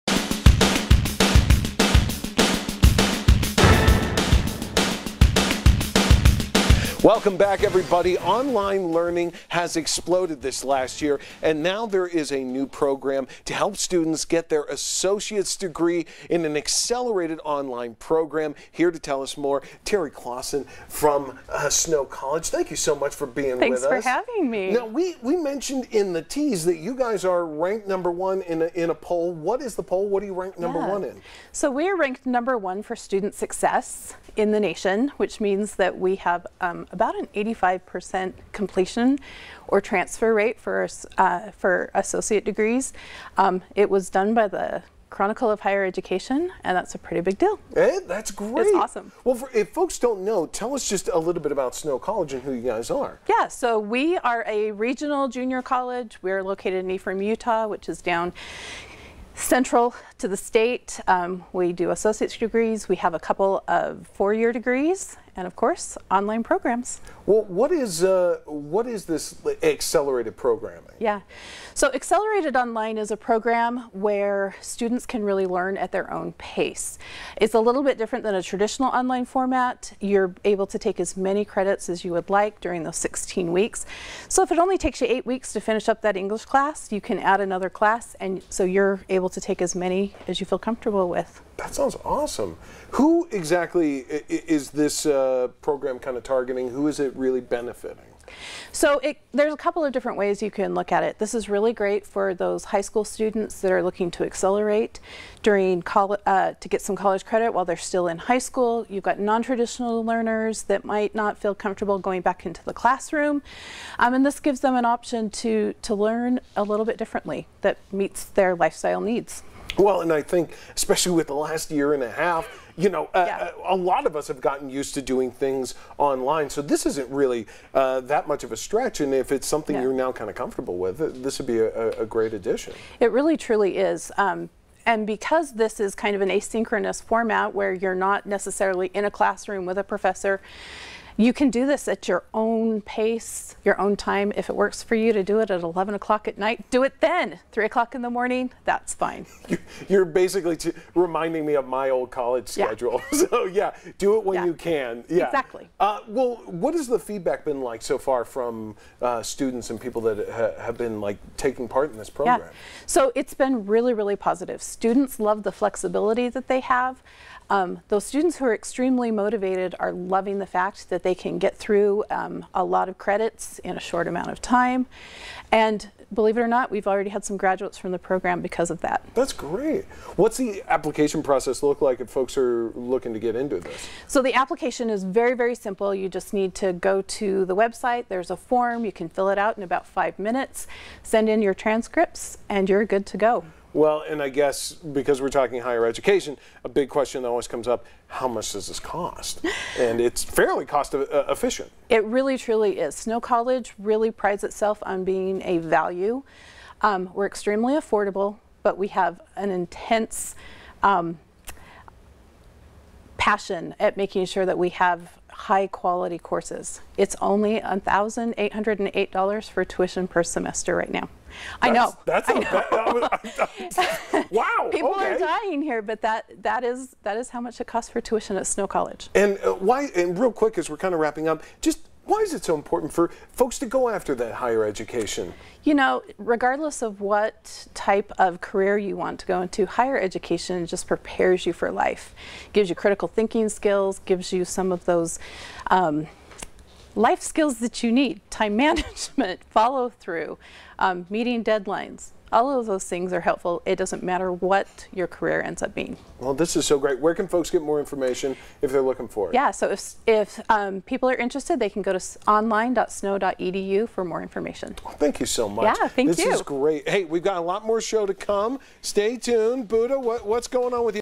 Snow College is #1 in the nation when it comes to credit transferability
Snow College launched Snow Online to provide students an accelerated path to earning their Associates Degree at their own time and pace.